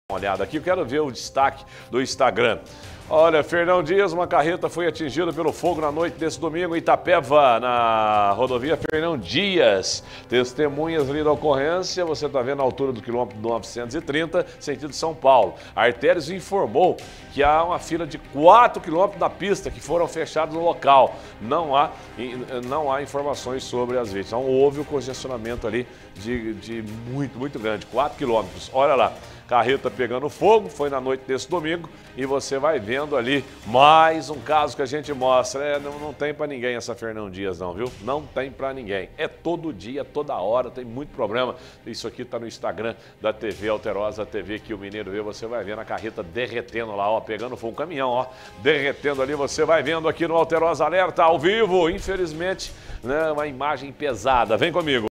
Carreta pega fogo em Itapeva
CARRETA PEGA FOGO EMITAPEVA | Uma carreta pegou fogo na rodovia Fernão Dias, em Itapeva. Veja os detalhes.